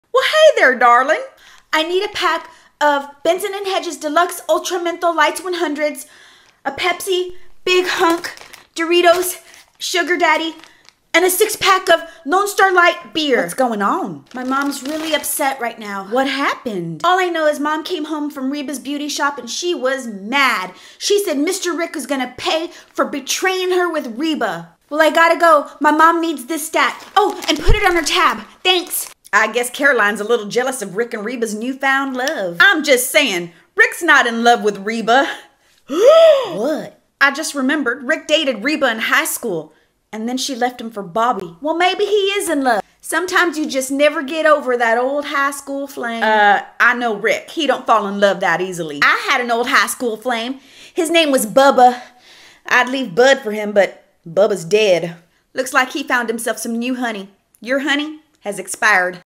Small Town Drama 80 for more
Small Town Drama Compilation Episode 80 is a soapopera dramaseries created by @Carmen Q Gollihar . It takes place in the 80s. misssherry is involved in a lovetriangle with Bobby and Rick. betrayal and deception is all over this smalltowns. You never know who you can trust .